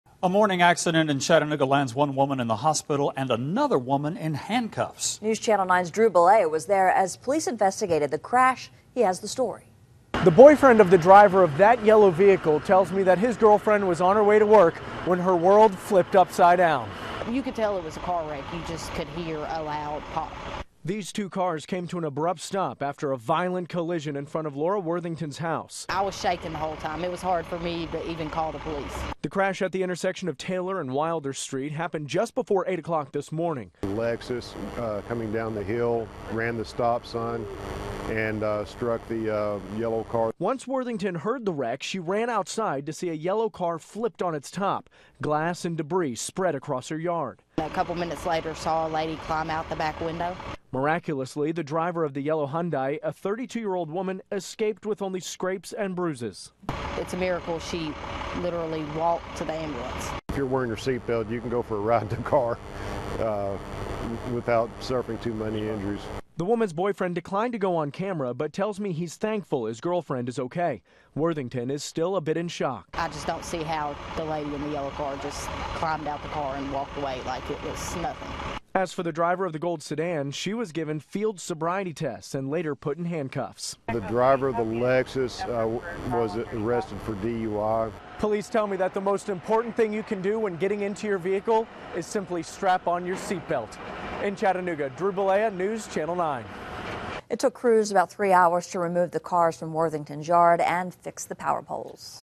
Crash near Amnicola Hwy Sends One to the Hospital
A morning accident in Chattanooga lands one woman in the hospital, and another woman in handcuffs.       You could tell it was a car wreck, said Laura Worthington, who was the first person there after the wreck happened, you could just hear a loud pop.The two cars came to an abrupt stop after a violent collision in front of Worthington's house. I was shaking the whole time, said Worthington, It was hard for me to even call the police. The crash at the intersection of Taylor and Wilder street happened just before 8:00am. The Lexus coming down the hill ran the stop sign and struck the yellow car, said Traffic Investigator, Joe Warren. Once Worthington heard the wreck, she ran outside to see the yellow car flipped on it's top, glass and debris spread across her yard. A couple minutes later I saw a lady climb out the back window, said Worthington.The driver of the yellow car, a 32 year old woman, escaped with only scraps and bruises. It's a miracle, said Worthington with a shocked look on her face, she literally walked to the ambulance.But Warren says he isn't too surprised, since the woman had her seat belt on. If you're wearing your seat belt, you can go for a ride in a car without suffering too many injuries, said Warren. The woman's boyfriend says he's thankful his girlfriend is okay. Worthington was still in shock. I just don't see how the lady in the yellow car got out and walked around like it was nothing. As for the driver of the gold sedan, she was given field sobriety tests and later put in handcuffs. The driver of the Lexus was arrested for DUI, said Warren.